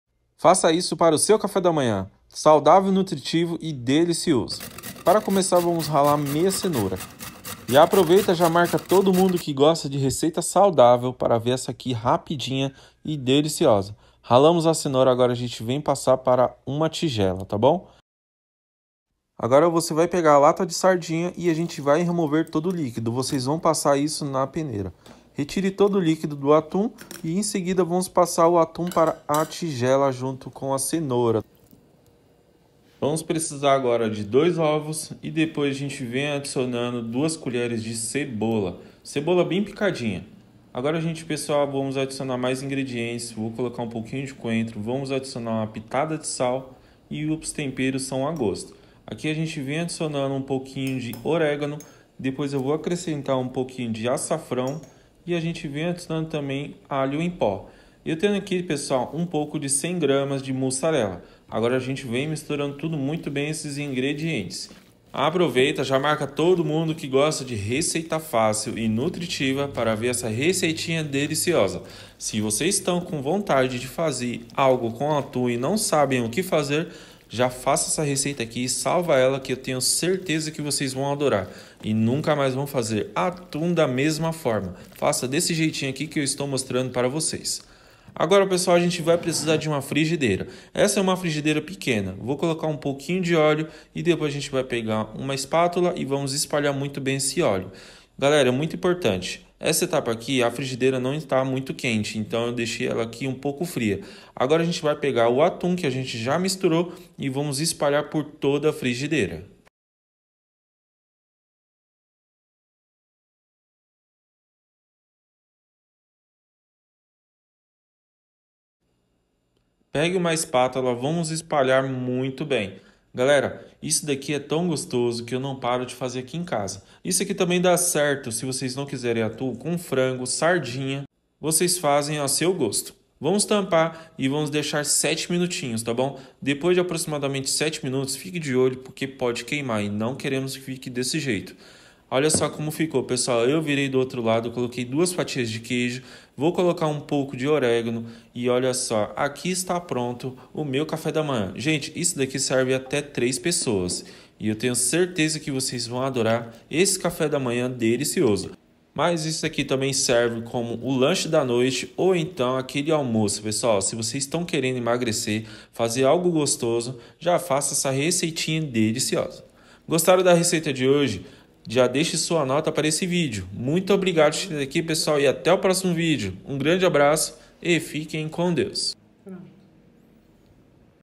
Não coma mais pão! Faça isso para o seu café da manhã !
Ingredientes:
1 Lata de atum
2 Ovos
1/2 cenoura
1 colher de cebola ralada
Coentro
Pitada de Sal
Orégano
Açafrão
Alho em Pó
100g mussarela
Coloque na frigideira e deixar fritar 8 minutos.
Poe duas fatias de mussarela e deixe derreter.